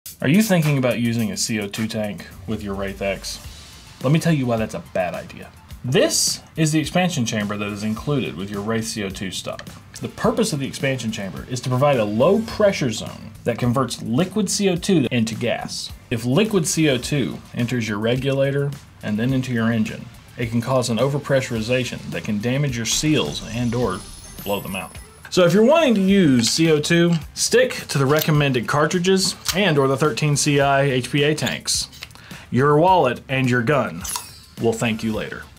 The Wraith X CO2 Expansion Chamber - What The Tech
Sam explains the purpose of the Wraith X CO2 expansion chamber and why you should never plug CO2 directly into your regulator.